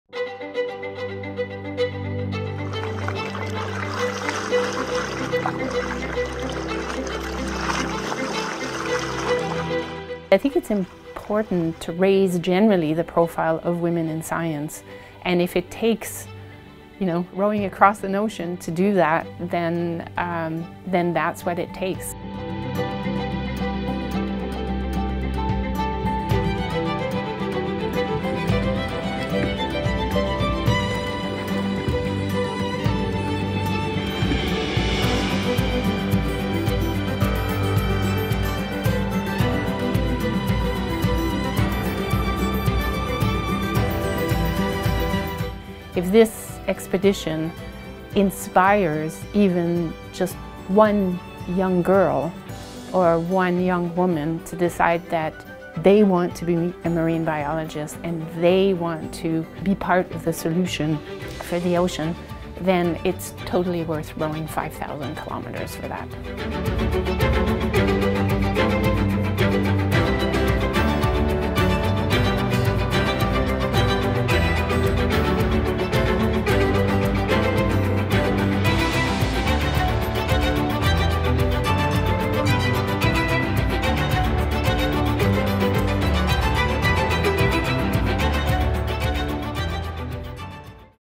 SFU marine biologist rowing across the Atlantic to raise funds for ocean conservation
It’s not every day that a former student invites you to row across the Atlantic Ocean but when SFU biological sciences professor Isabelle Côté got the call, she didn’t hesitate to say yes—inspired by the challenge and the journey’s cause of supporting ocean conservation.

In December Côté and three other marine biologists will take part in one of the toughest races in the world— the Talisker Whisky Atlantic Challenge—to raise money for marine conservation charities.

The annual endurance race is designed to give up to 30 teams the opportunity to challenge themselves against Mother Nature through the experience of crossing the ocean in a rowing boat.